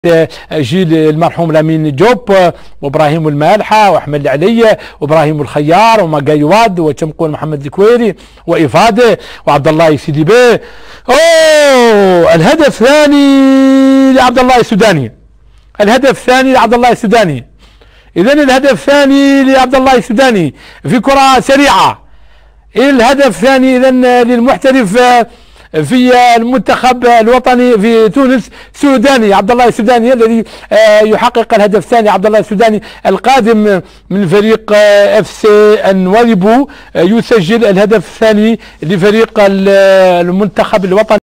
الهدف الثاني للمنتخب الوطني في شباك الكونغو، توقيع النجم الحسن العيد - قناة الموريتانية
بهذا الهدف يتقدم منتخبنا الوطني المرابطون على منتخب الكونغو بهدفين مقابل هدف وكان الهدف الأول على يد نجم المنتخب الوطني الحسن العيد
قناة الموريتانية